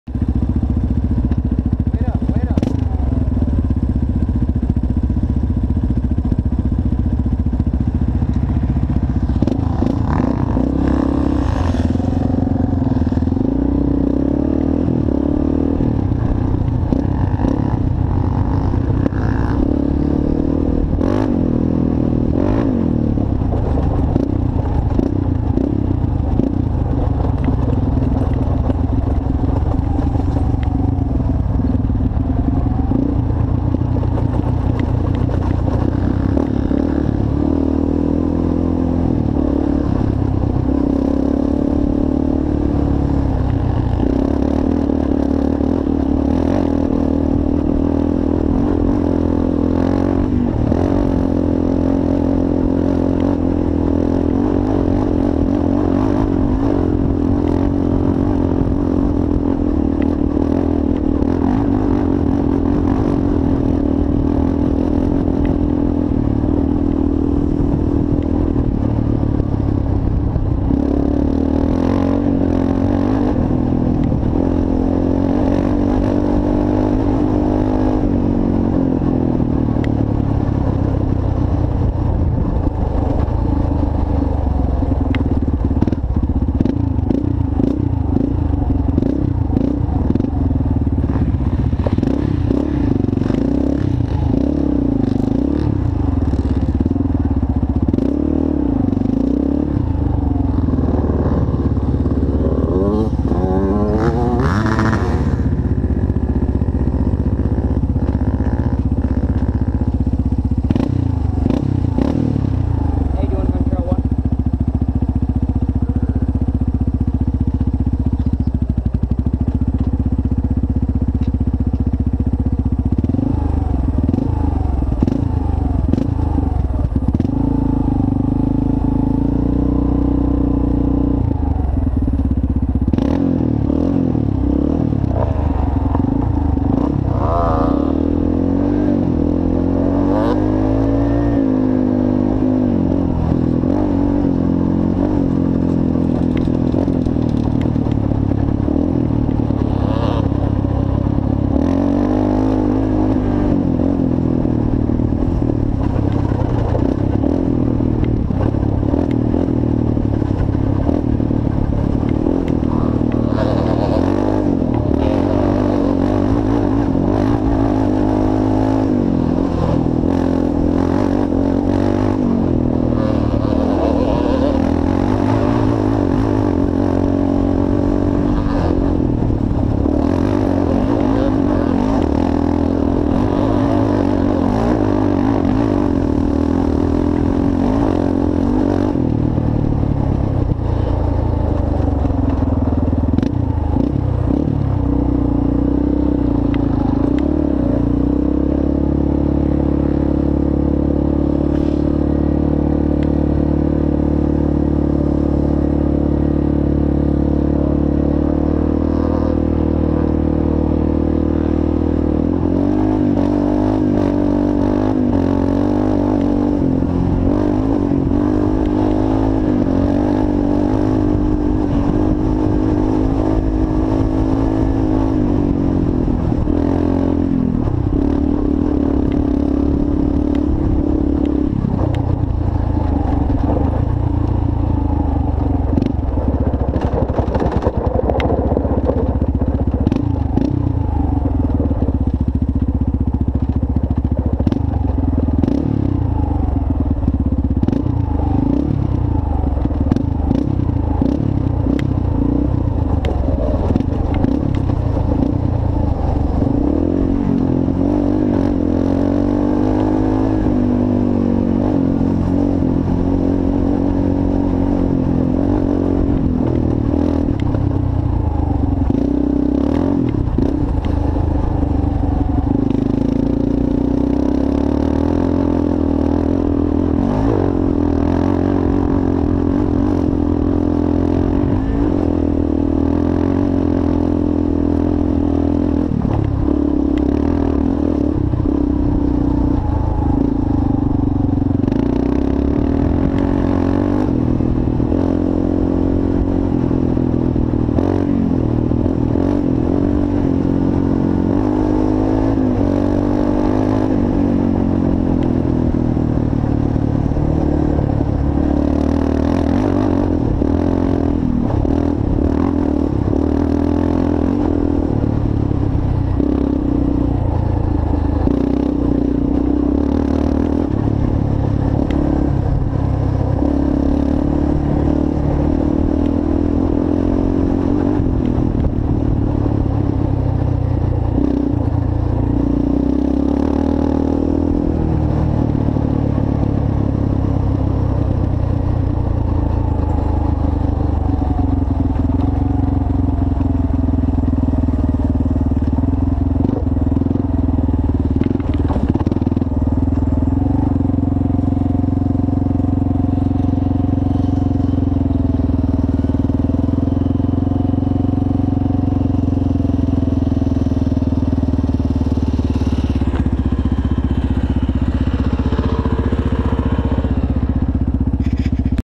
Metcalf Riding 350 Raptor (7-7-13)
Me on my 350 raptor riding at metcalf motorcycle park riding trail 2, and trail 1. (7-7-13)